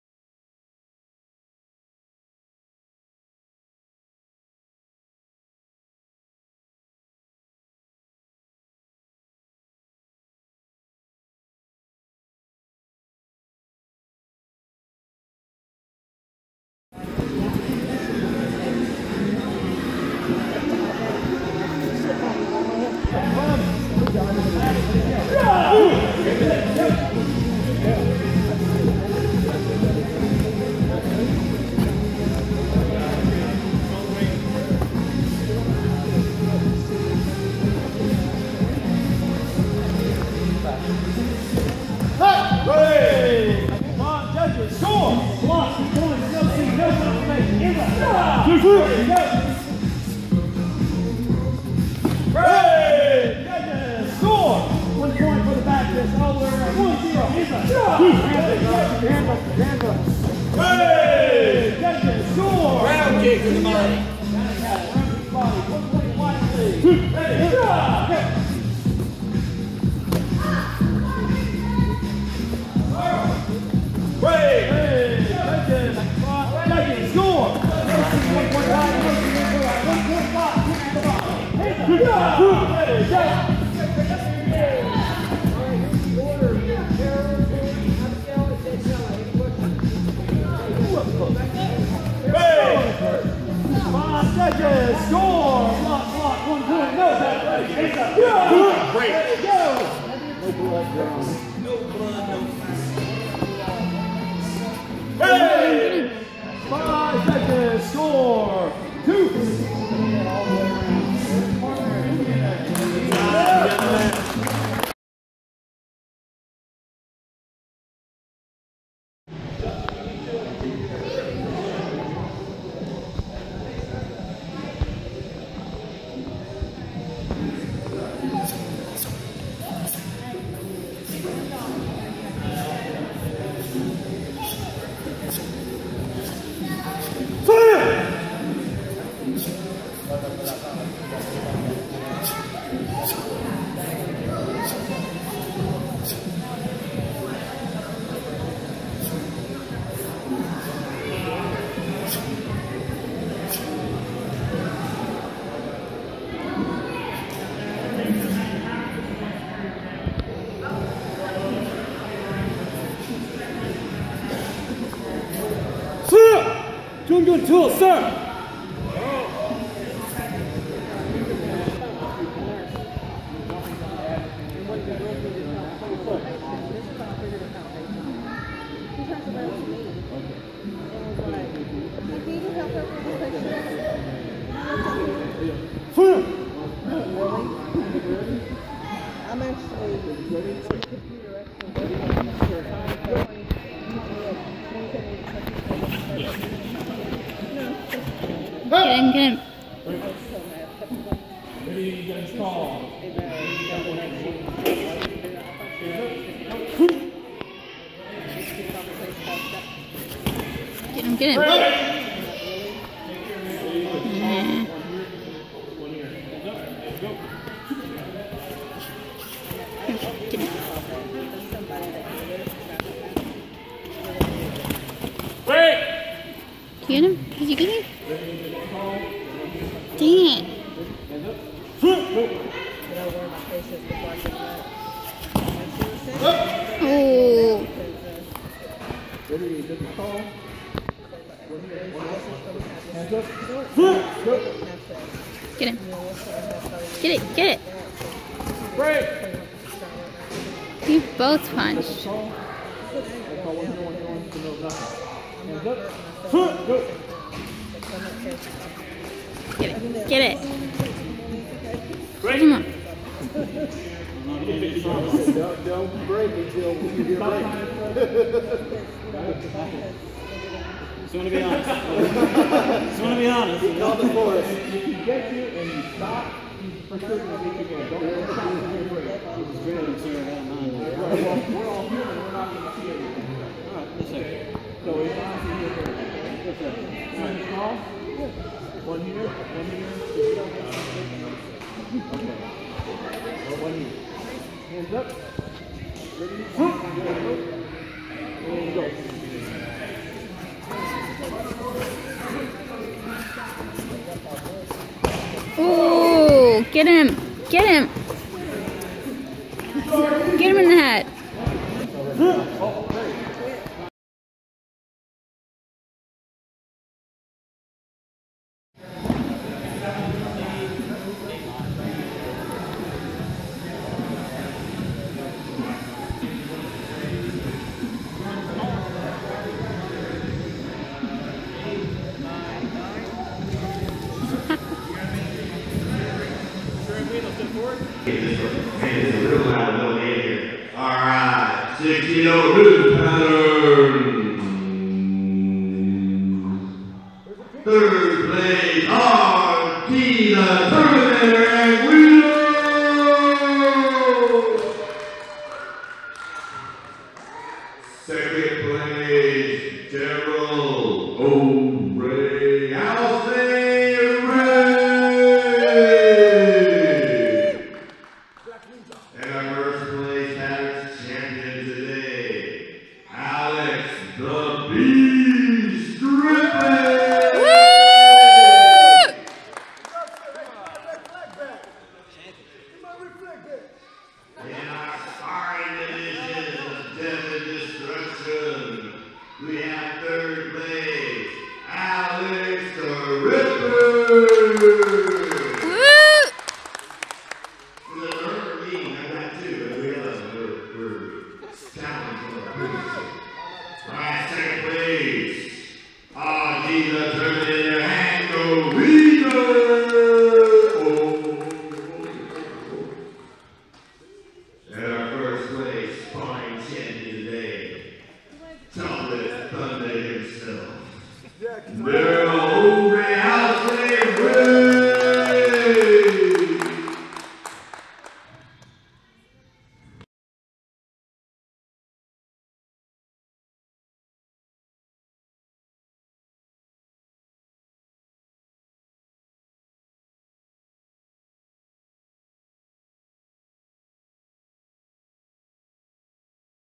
UTA Spring Fling Regional Tournament Spring 2016
Alex Stripling Competing in the UTA Spring 2016 Regional Spring Fling Tournament
From:
Fayetteville Martial Arts
Master Randy Edwards